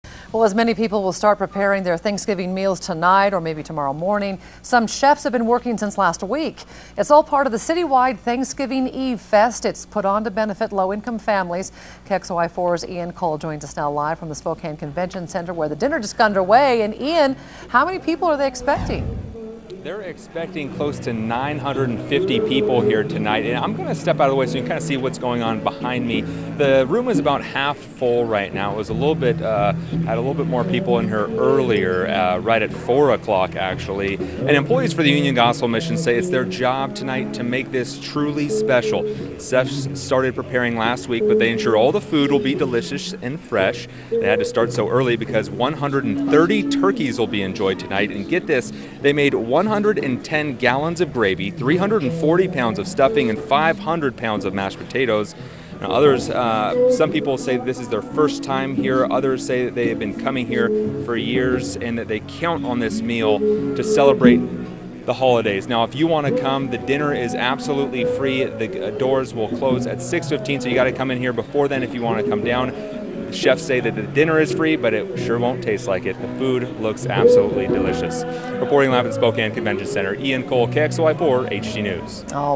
Union Gospel Mission Puts On Thanksgiving Eve Feast
Many people will start preparing their Thanksgiving meals Wednesday night or early Thursday morning, but at the Union Gospel Mission their chefs have been cooking since last week. KXLY4's Ian Cull reports.